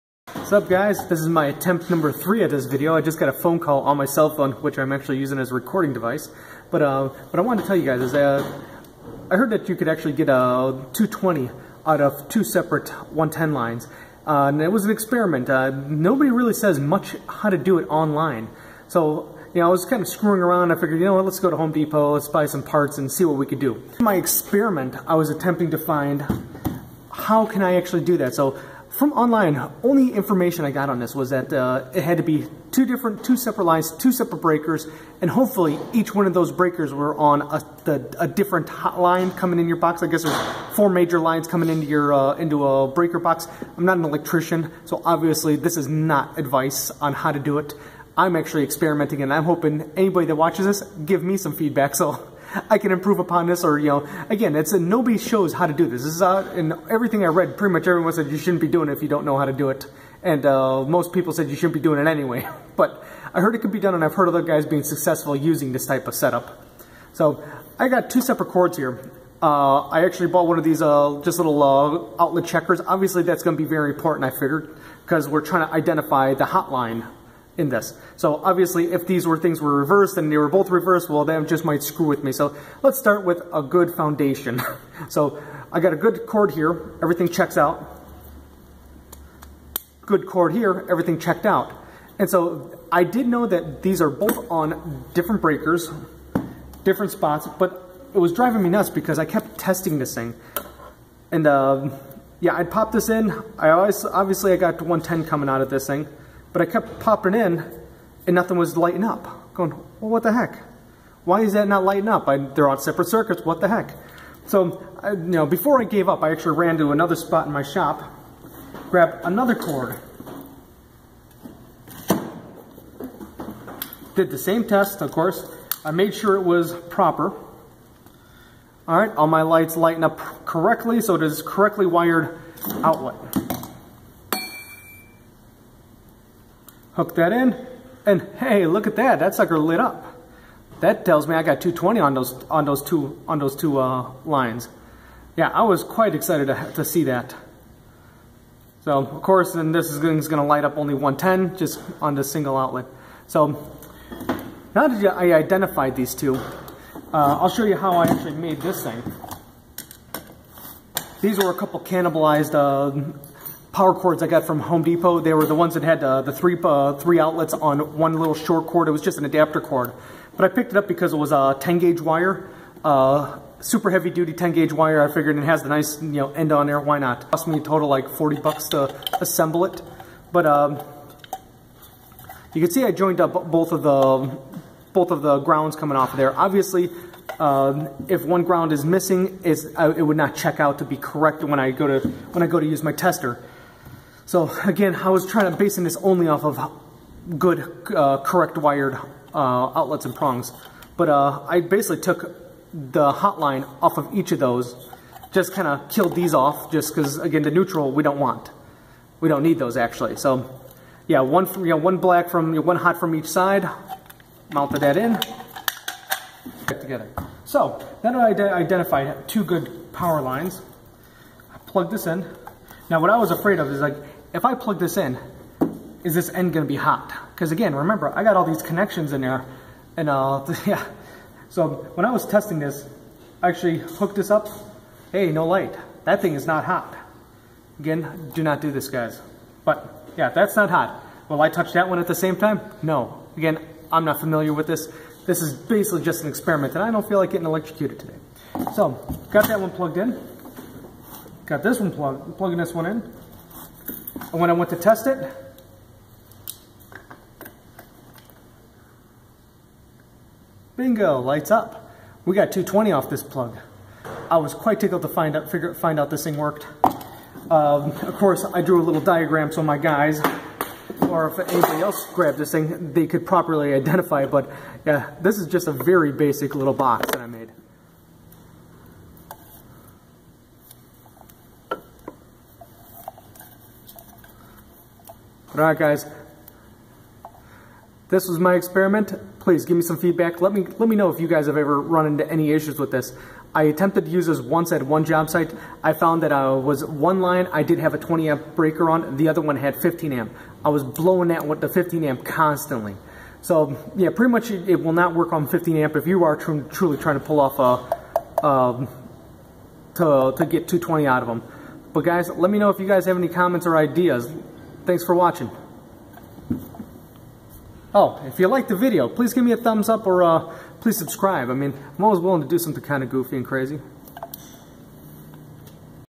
Experiment convert 110v to 220v from two outlets
Warning, This is only an experiment for viewing purposes only! By watching this You agree that "Joshua Burton" is not held responsible for your actions.

This is my successful experiment to make an adapter to convert two 120v outlets to 220v!
With that said I used two 12ga. heavy duty extension cords and triple checked my connections and wiring.